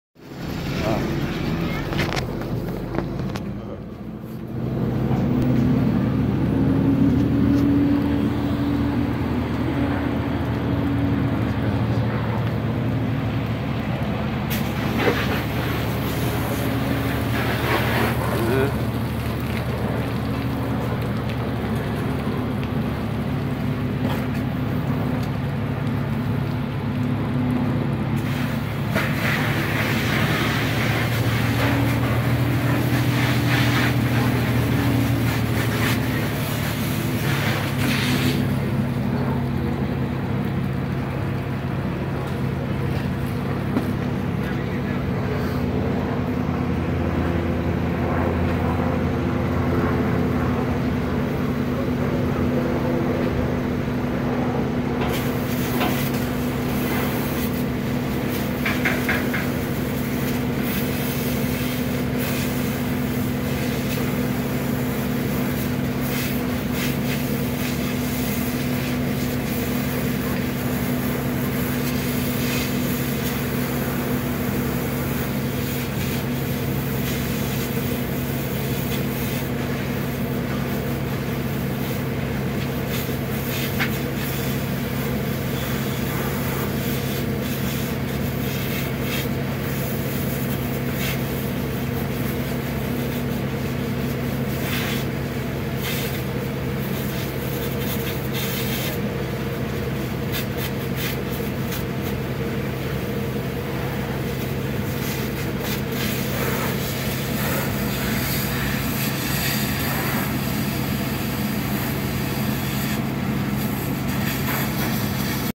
Detroit car fire on hoist, fire department responds
Detroit, MI fire department responds to a car fire.  My friend was welding underneath and didn't realize the carpet had ignited above.